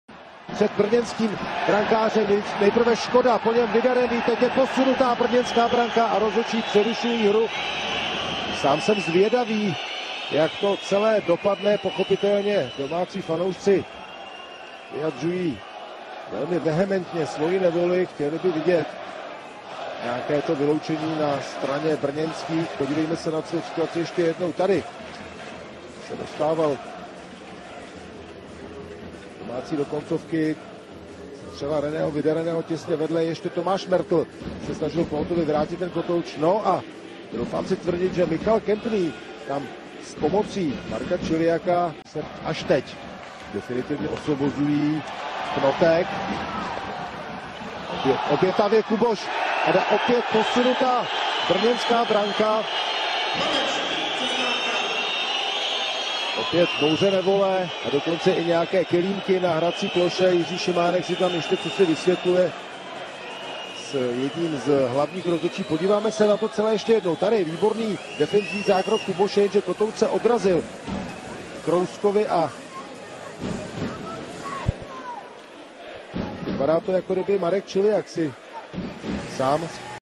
DK Čiliak Marek 03.10.2013
Jednání brankáře klubu Kometa Brno Marka Čiliaka z utkání Mountfield HK - Kometa Brno, které předseda Disciplinární komise ELH řešil na základě podnětu Komise rozhodčích ČSLH. Předseda Disciplinární komise ELH po zhlédnutí videozáznamu shledal, že brankář v obou projednávaných případech (tj. čas 44.58 a 53.28) při dokončení brankářského zákroku zasahuje bruslí brankovou konstrukci, přičemž zejména v situaci z času 53.28 jde o očividné kopnutí do brankové konstrukce s cílem tuto vychýlit.
Vzhledem k tomu, že byl v jednání hráče prokázán úmysl vychýlit brankovou konstrukci ze svého postavení, a to zejména ve druhé situaci z času 53.28, uložil předseda Disciplinární komise ELH Karel Holý hráči napomenutí za naplnění skutkové podstaty disciplinárního přestupku §29 Jednání proti fair play, a to s přihlédnutím ke skutečnosti, že za dvojí vychýlení branky v krátkém časovém úseku, přičemž v obou případech se jednalo o stejnou stranu brankové konstrukce, nemohl pouze brankář Čiliak svým aktivním a cíleným pohybem, nýbrž i evidentní vadné ukotvení brankové konstrukce. (zdroj videa: BPA Sport Marketing)